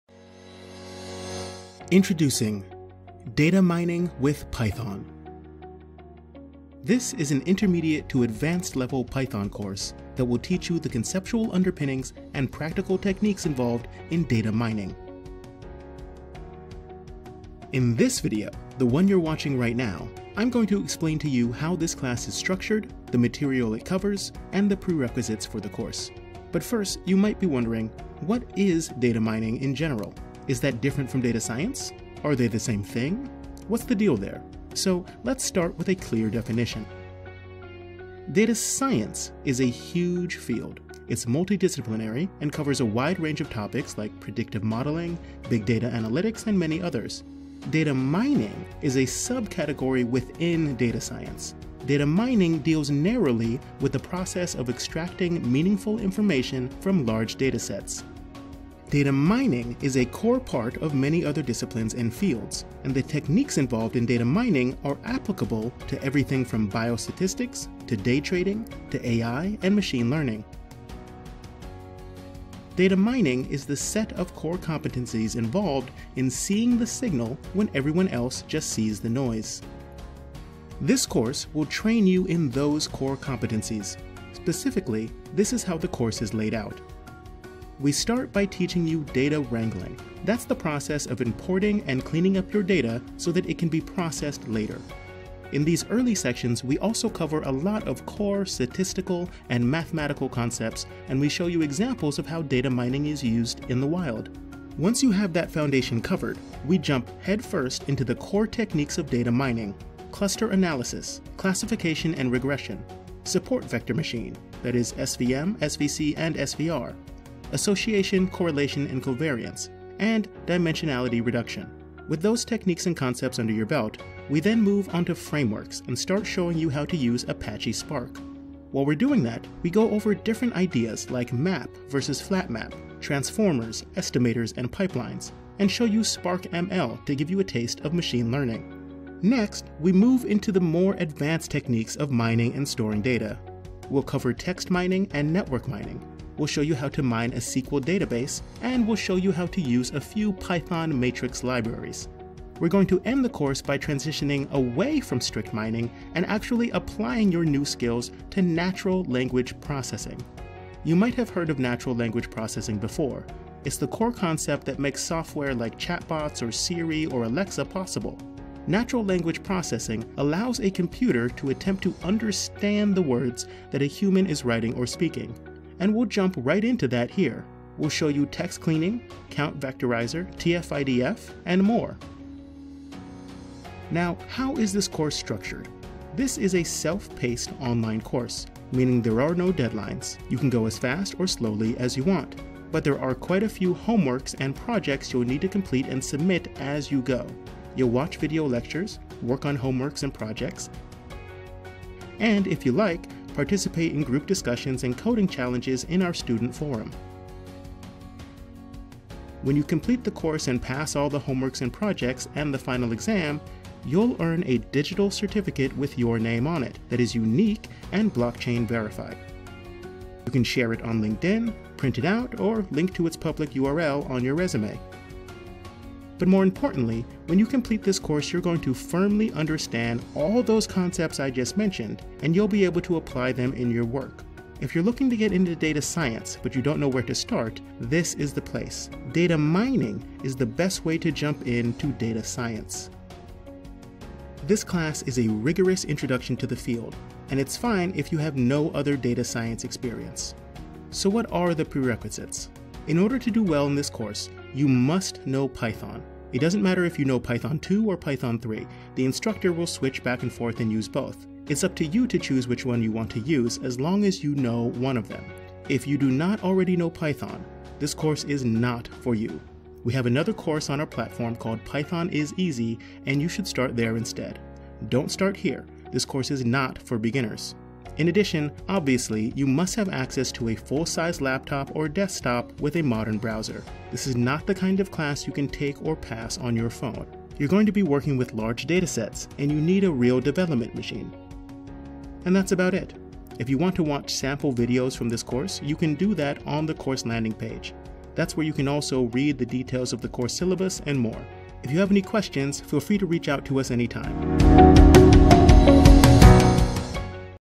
What is Data Mining Vs. Data Science ? and Why you should learn them ?
Introduction to Data Mining/Data Science and why you should learn them. This is a future free BBDS course.